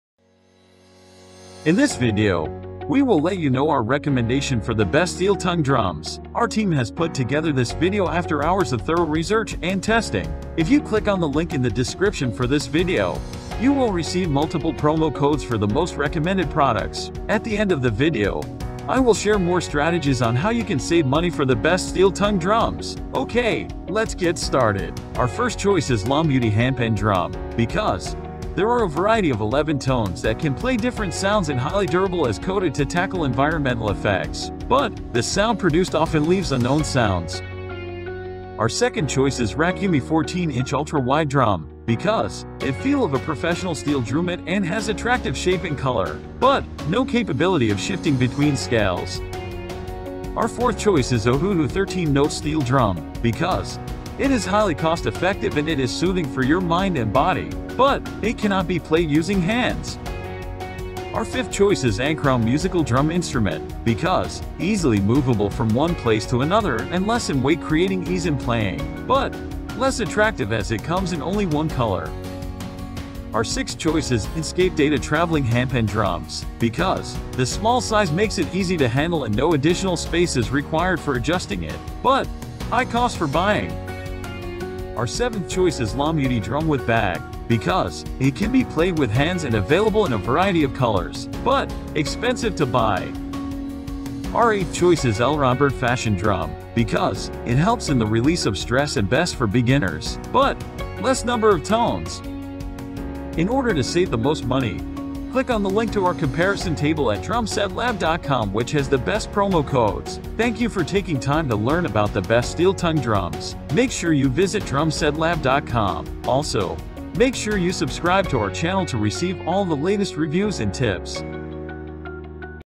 BEST STEEL TONGUE DRUMS
We’re going to discuss the pros and cons of All The BEST STEEL TONGUE DRUMS

Here are the details:
1. Lomuty Handpan Drum
2. Rakumi 14 inch Ultra Wide Drum
3. Ohuhu 13 Notes Steel Drum
4. ANCROWN Musical Drum Instrument
5. Inscape Data Travelling Handpan Drums
6. LOMUTY Drum with Bag
7. Lronbird Fashion Drum